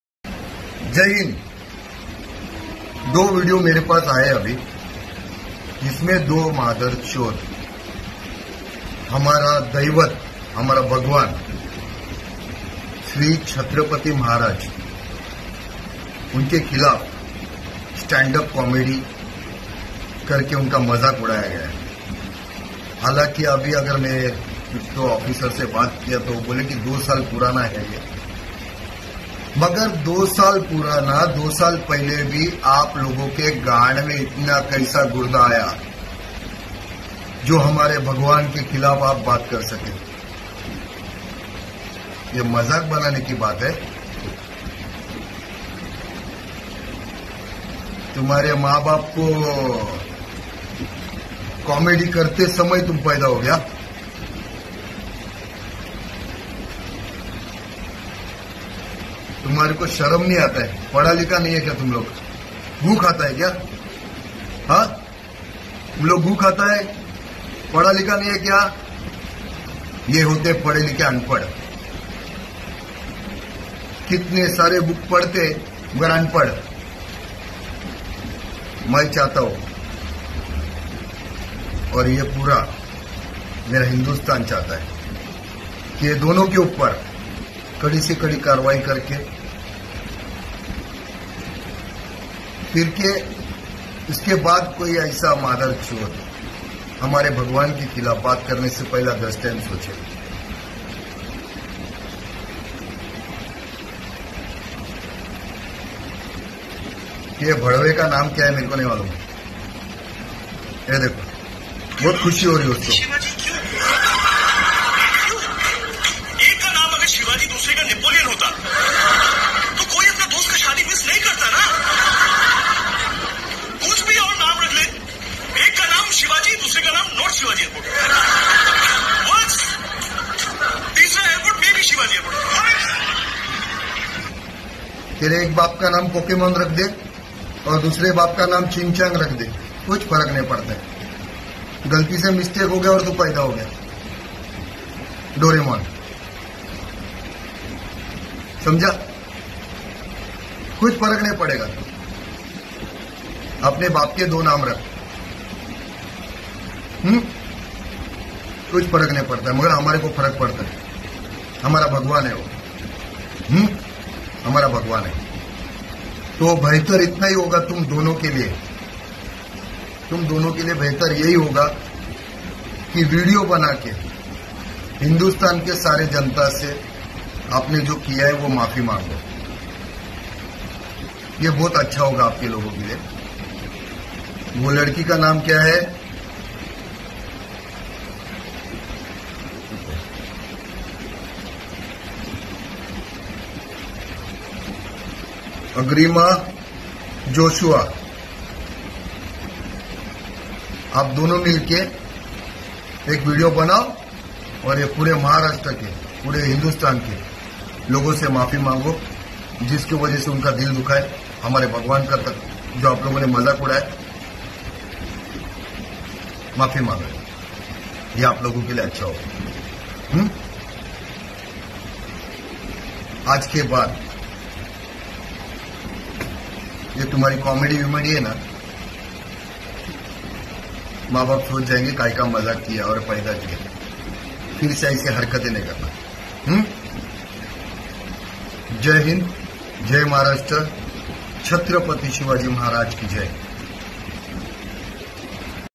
Hindustani Bhau's reaction on Stand up comedian Saurabh Gosh and Agrima Joshua Viral video
hindustanibhau standupcomedy Tiger Group Maharashtra